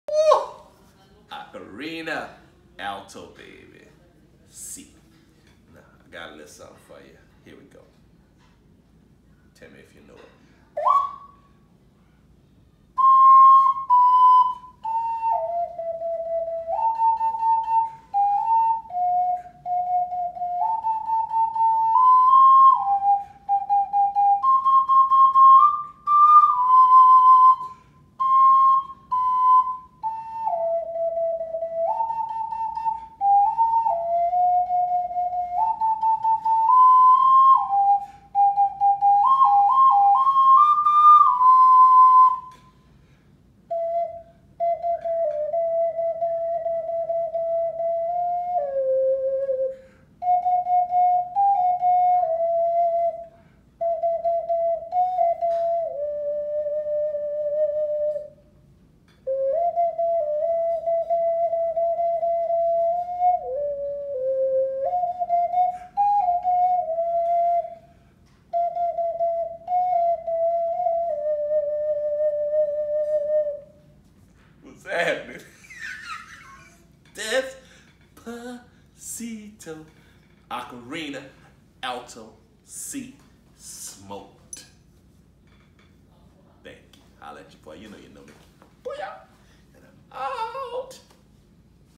Clarinet player try to play the Ocarina
Clarinet player try to play the Ocarina Guess what's this top Latin hit?
Smoked Ocarina